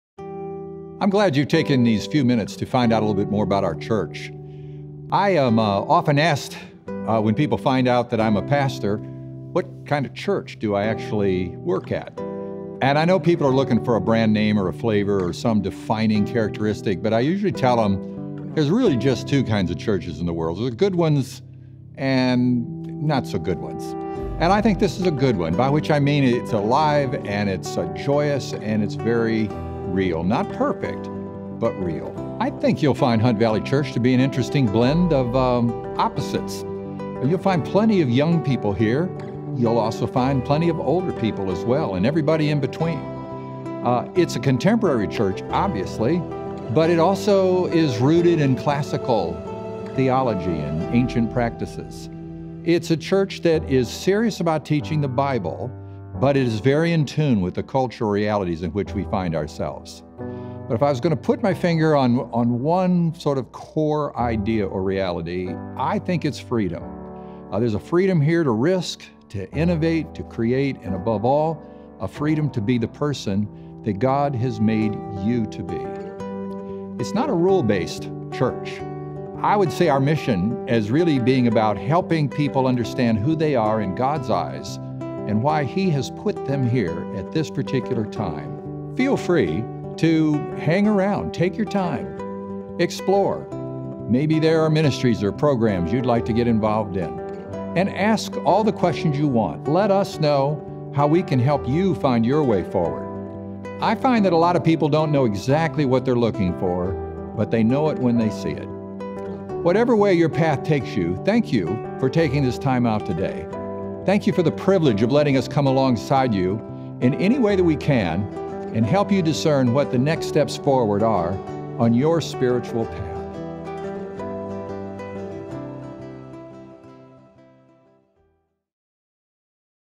Welcome From Pastor Frank
Pastor Frank tells us a little about what makes Hunt Valley Church unique and a good fit for you.